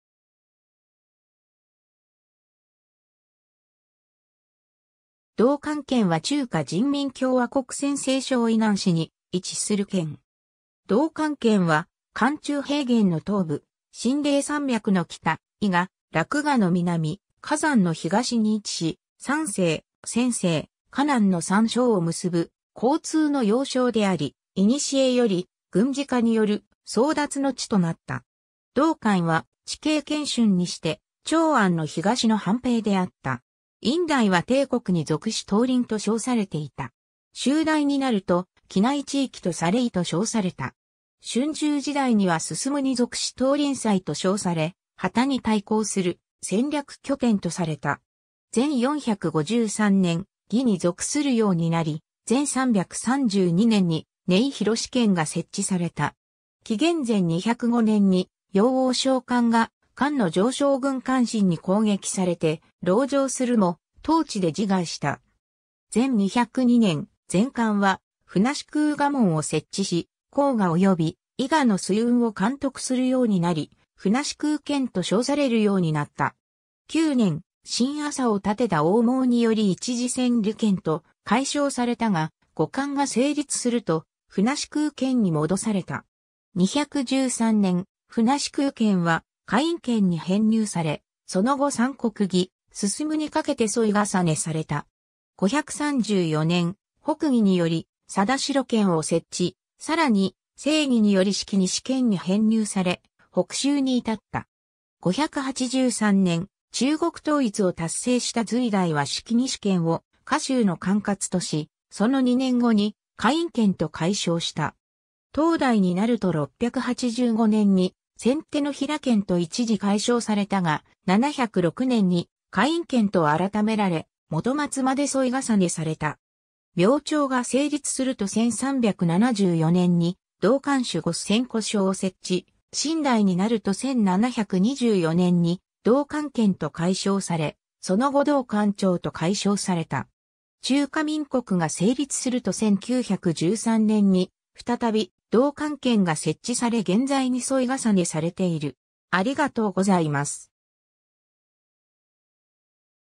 潼関県
潼関県（どうかん-けん）は中華人民共和国陝西省渭南市に位置する県。
 潼関県は関中平原の東部、秦嶺山脈の北、渭河、洛河の南、華山の東に位置し、山西、陝西、河南の3省を結ぶ交通の要衝であり、古より軍事家による争奪の地となった。
潼関は地形険峻にして、長安の東の藩屏であった。
 殷代は定国に属し桃林と称されていた。
周代になると畿内地域とされ渭汭と称された。
春秋時代には晋に属し桃林塞と称され秦に対抗する戦略拠点とされた。
前453年、魏に属するようになり、前332年に寧秦県が設置された。
紀元前205年に雍王章邯が漢の上将軍韓信に攻撃されて、籠城するも当地で自害した。
 前202年、前漢は船司空衙門を設置し、黄河及び渭河の水運を監督するようになり、船司空県と称されるようになった。
9年（始建国元年）、新朝を立てた王莽により一時船利県と改称されたが、後漢が成立すると船司空県に戻された。
213年（建安18年）、船司空県は華陰県に編入され、その後三国魏、晋にかけて沿襲された。
534年（永熙3年）、北魏により定城県を設置、更に西魏により敷西県に編入され北周に至った。
 583年（開皇3年）、中国統一を達成した隋代は敷西県を華州（後に京兆郡）の管轄とし、その2年後に華陰県と改称した。
唐代になると685年（垂拱元年）に仙掌県と一時改称されたが、706年（神龍2年）に華陰県と改められ元末まで沿襲された。
 明朝が成立すると1374年（洪武7年）に潼関守御千戸所を設置、清代になると1724年（雍正2年）に潼関県と改称され、その後潼関庁と改称された。
 中華民国が成立すると1913年に再び潼関県が設置され現在に沿襲されている。